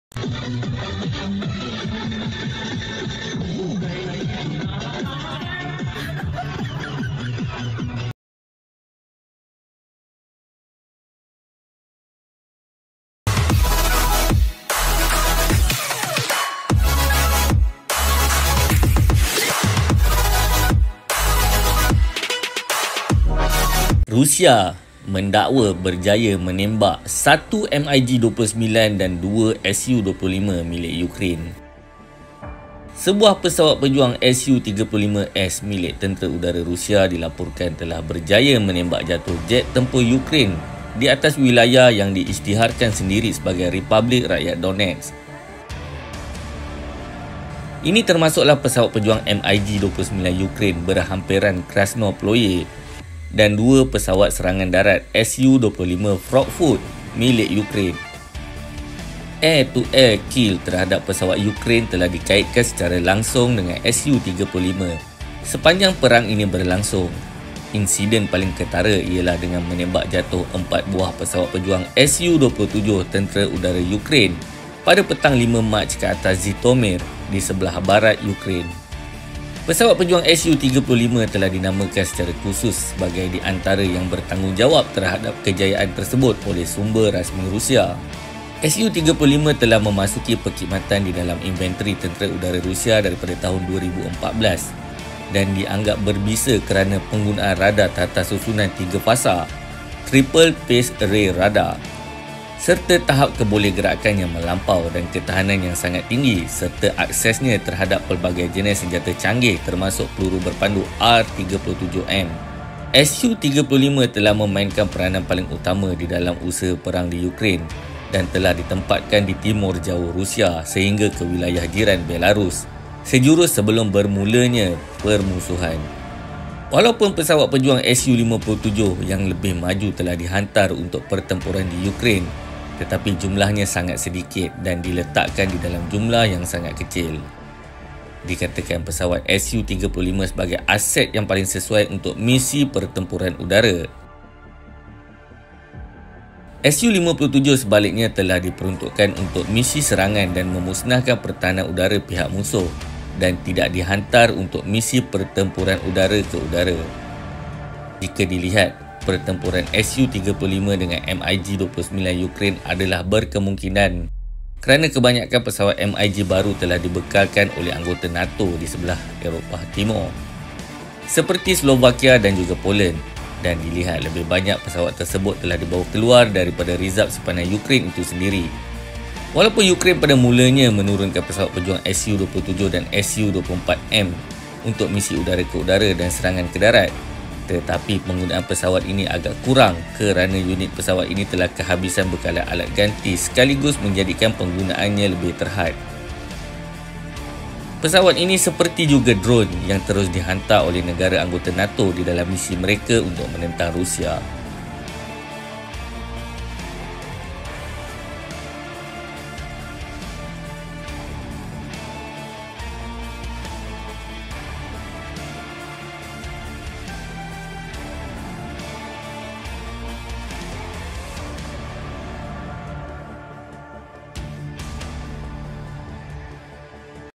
SU35 "AIR TO AIR KILLS" MIG-29&SU25 FROGFOOT?
Su-35S Rusia Claim "Air To Air Kills" di Donetsk: Satu MiG-29 dan Dua Su-25

Sebuah pesawat pejuang Su-35S milik Tentera Udara Rusia dilaporkan mendakwa berjaya menembak jatuh jet tempur Ukraine di atas wilayah yang diisytiharkan sendiri sebagai Republik Rakyat Donetsk,ini termasuk pesawat pejuang MiG-29 Ukraine berhampiran Krasnoploye dan dua pesawat serangan darat Su-25 frogfoot.